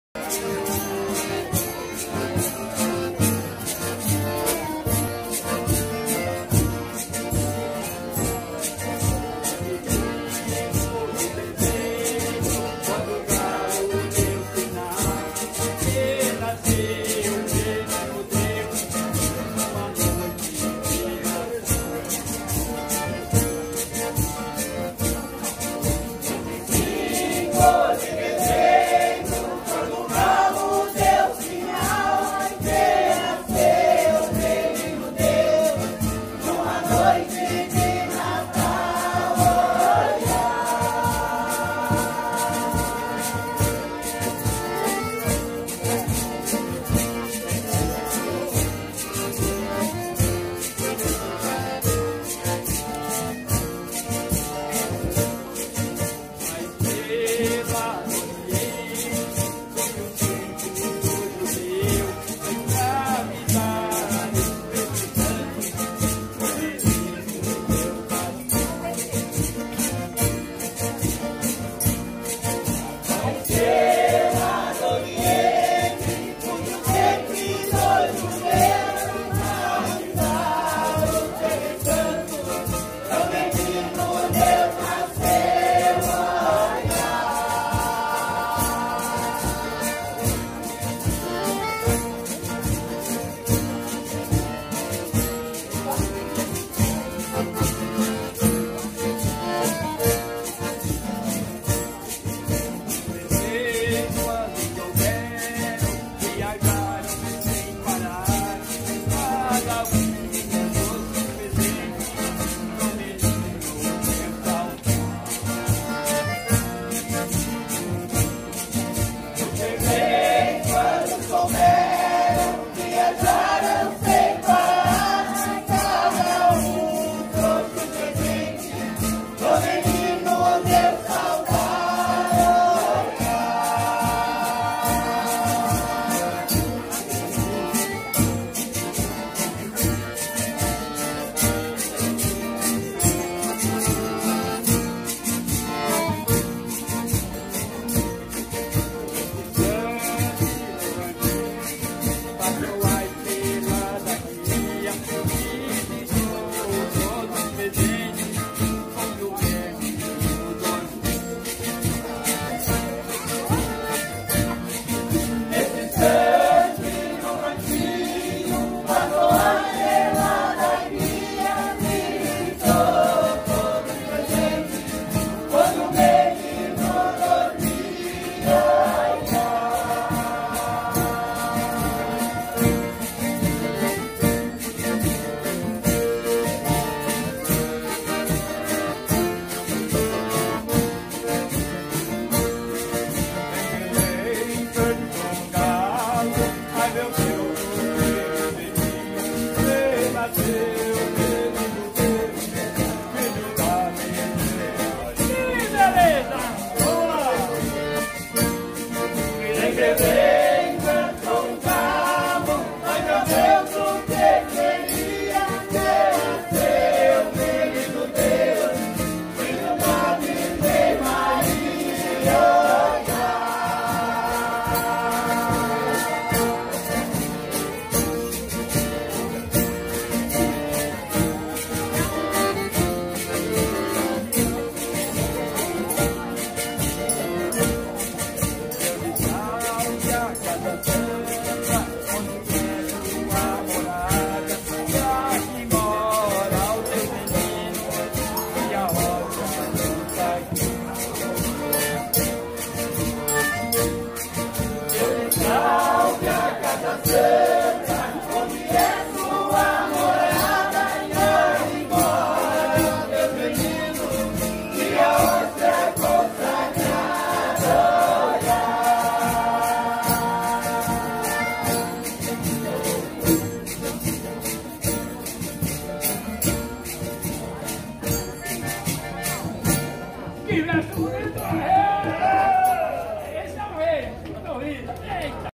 Casa Iluminada Blumenau SC. Terno de Reis. Hino de Reis.Com Grupo Cantoria Campos Novos Blumenau sc.
Gente quem poder deve pelo menos uma vez na vida vizitar esta Casa Iluminada aqui em Blumenau sc.